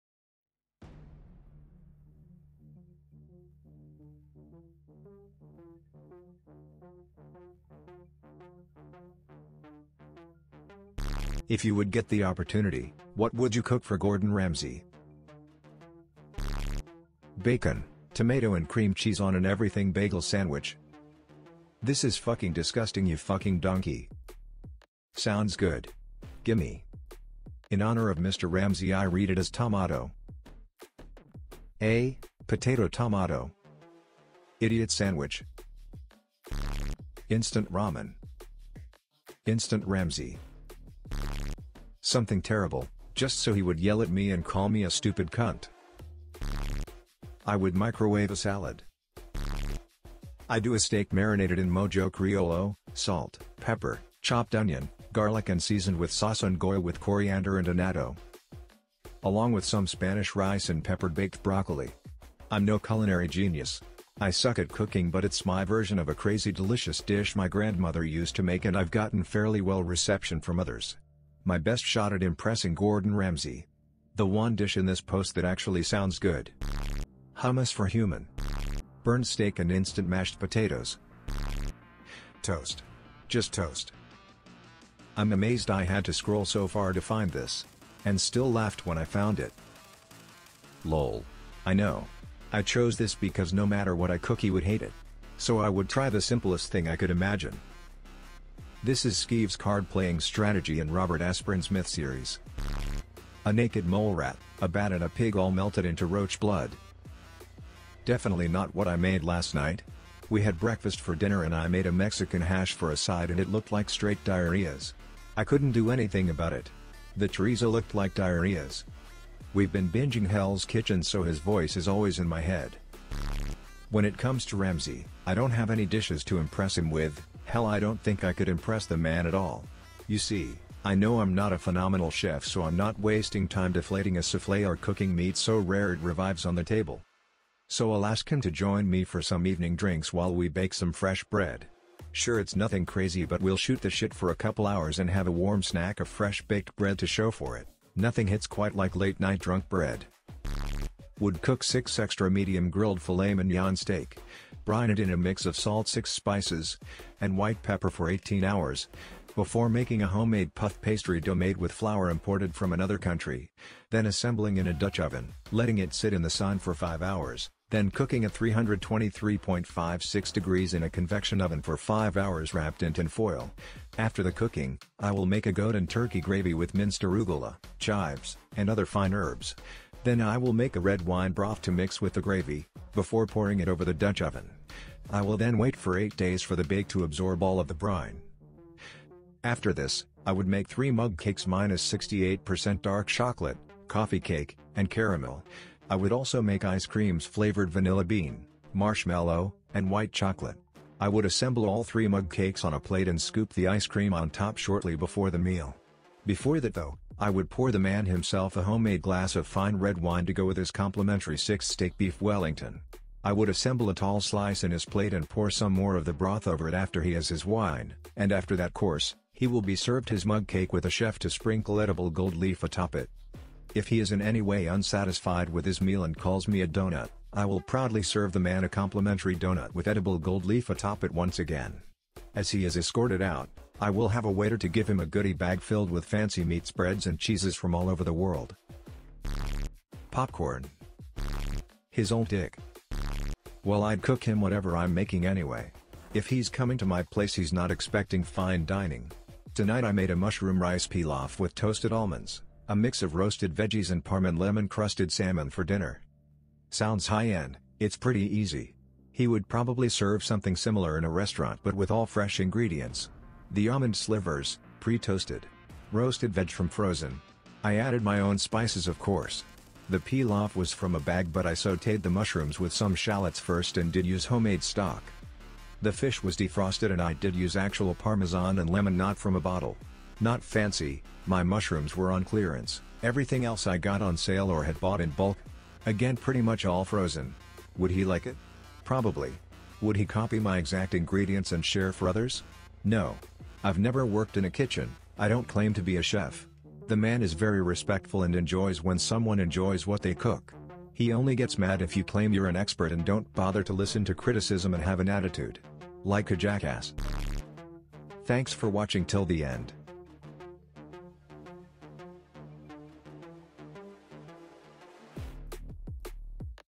IF YOU WOULD GET THE OPPORTUNITY WHAT WOULD YOU COOK FOR GORDON RAMSEY? r/AskReddit|Reddit Stories
r/AskReddit

Source and Credit:
💀  Written By : [SERIOUS] What is the Creepiest or most Unexplained thing that’s happened to you that you still think about to this day?